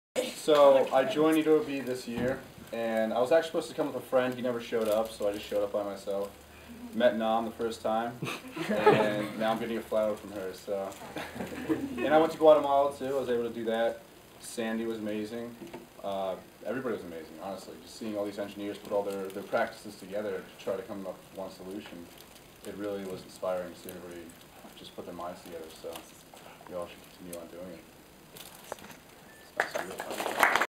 EWB Senior Speeches 2010: Colin
Colin explains what EWB means to him and reflects on his time at EWB Rutgers.